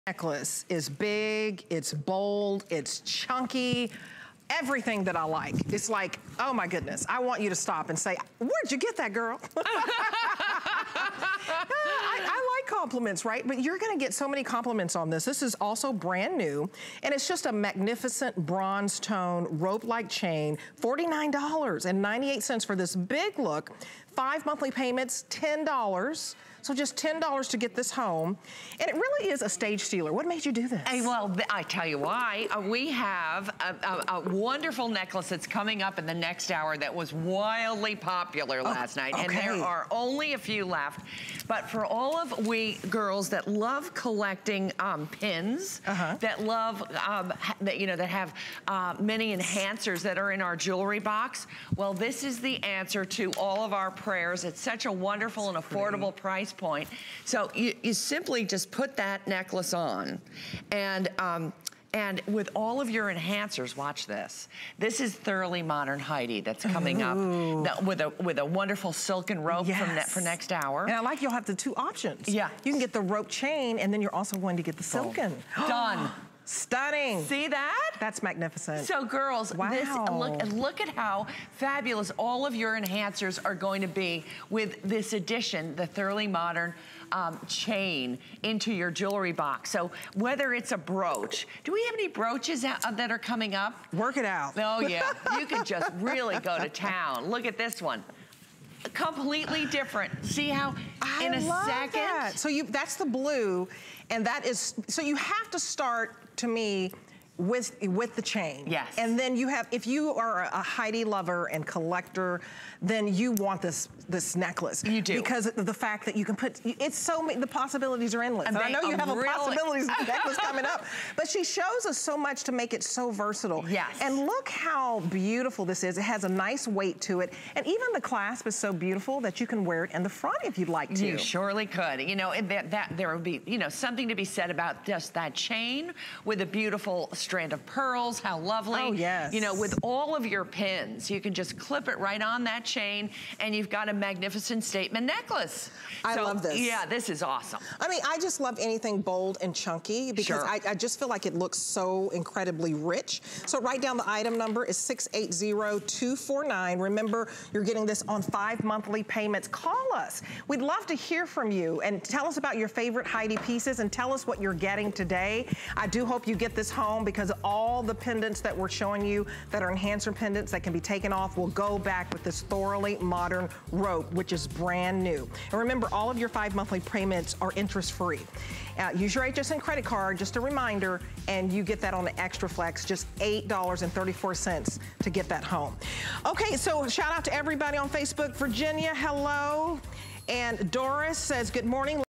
Heidi Daus "Thoroughly Modern" RopeLink Chain Necklace
Big, bold and chunky. Outshine the rest with this pretty, bronzetone ropelink chain necklace that steals the stage. Wear it alone, layer it with other necklaces or add an enhancer pendant for a unique look that's all your own.

Crystal Information

    This piece contains clear crystals